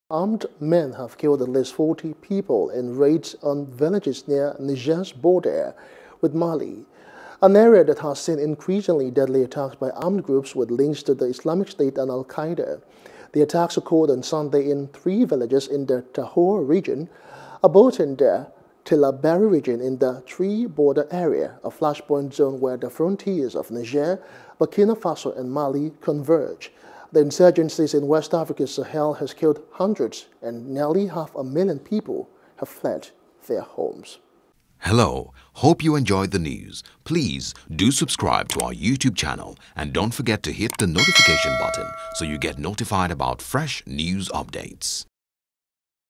40 Killed In Fresh Attack In Niger Near Mali Border | AFRICAN
Armed men have killed at least 40 people in raids on villages near Niger's border with Mali.